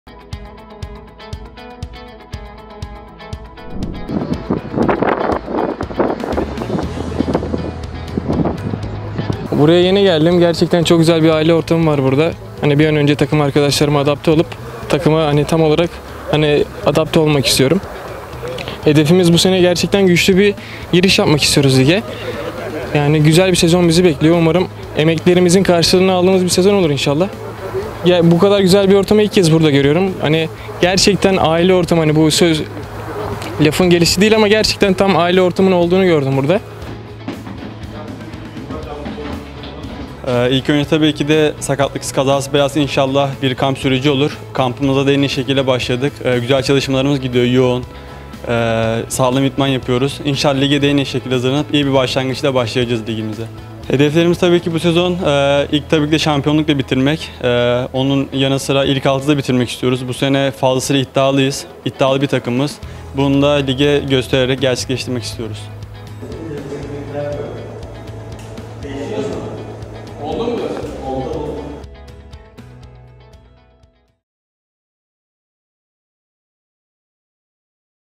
🎙️"ÇOK GÜZEL BİR AİLE ORTAMI İLE KARŞILAŞTIM"
Yeni transferlerimizden Bertuğ Bayar ile Yusuf Can Esendemir, yeni sezon çalışmaları hakkında Altınordu TV'ye konuştu.